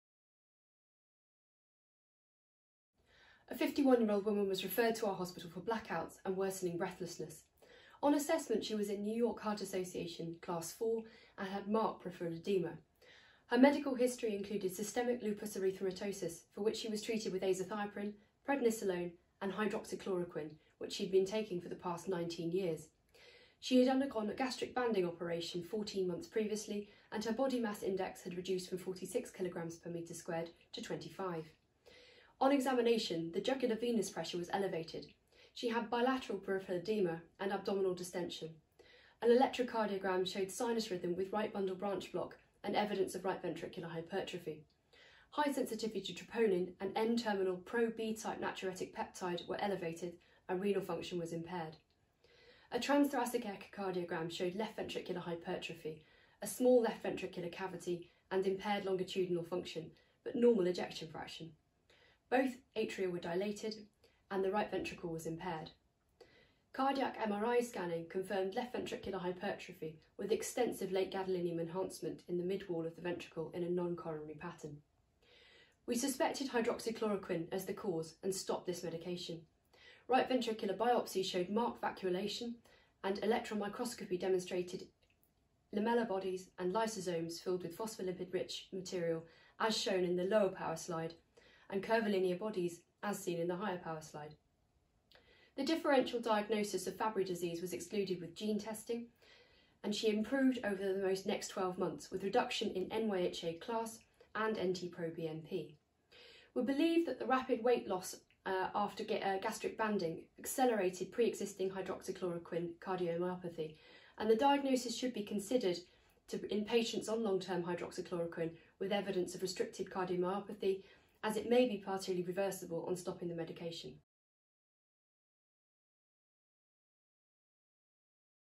Hydroxychloroquine-induced cardiomyopathy precipitated by gastric banding
Cardiomyopathy and hydroxychloroquine: accelerated after gastric banding

Read the full Clinical Picture, Hydroxychloroquine-induced cardiomyopathy accelerated after gastric banding